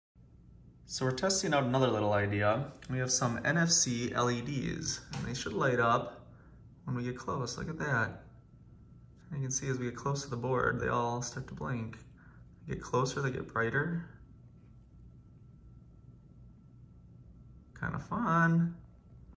Cool Project Idea NFC LED Tags
In preparation for the East Troy Holiday Light show, we came across another cool project idea. We found these little NFC LED tags on Adafruit. I believe the NFC tags are technically made for fingernails, but I'm sure we can put them to other uses!

For anyone interested in these products